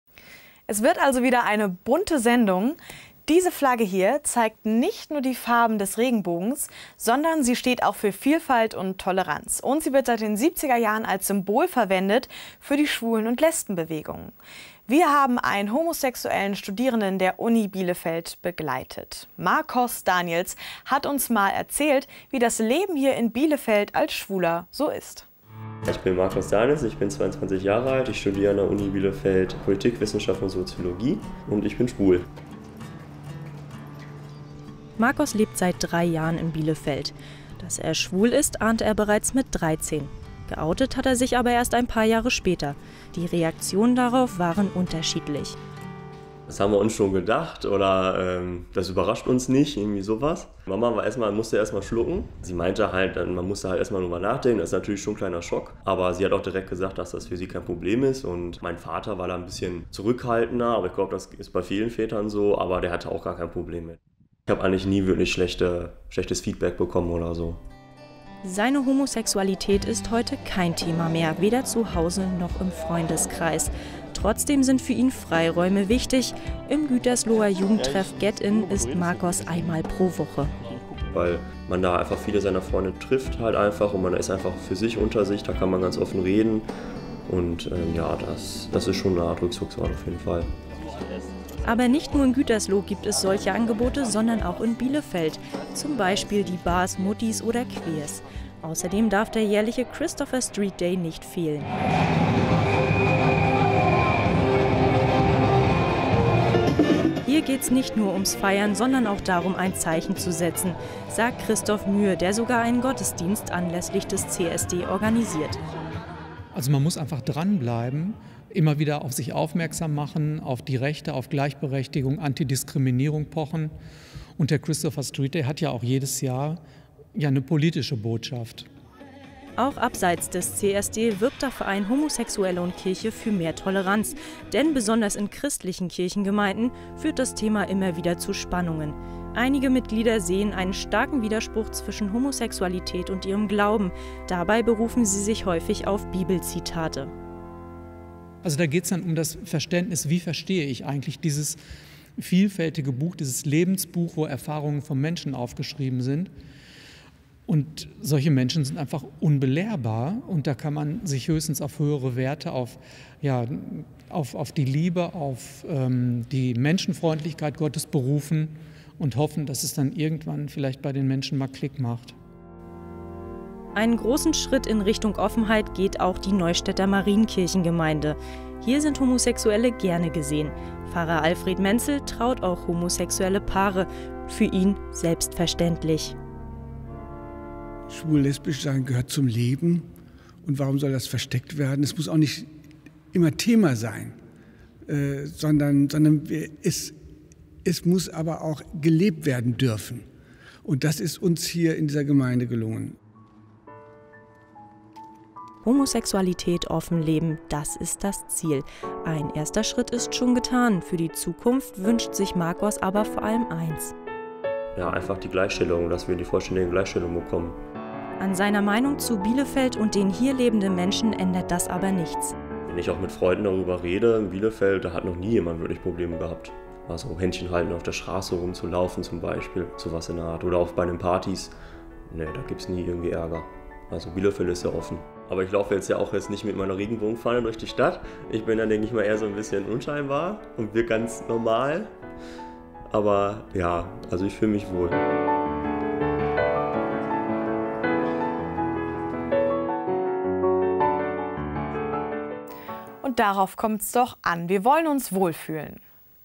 Homosexuell in Bielefeld - Campus TV Uni Bielefeld (Folge 103)
Die Regenbogenflagge schmückt jedes Jahr die Straßen Bielefelds zum Christopher Street Day, sie wird seit den 1970ern von der Lesben- und Schwulenbewegung als Symbol verwendet, sie steht unteranderem für Vielfalt und Toleranz.
Doch wie sieht es mit der Akzeptanz in Bielefeld aus?
Wir haben einen homosexuellen Studierenden der Uni Bielefeld begleitet, er erzählt uns, wie das Leben in Bielfeld so ist.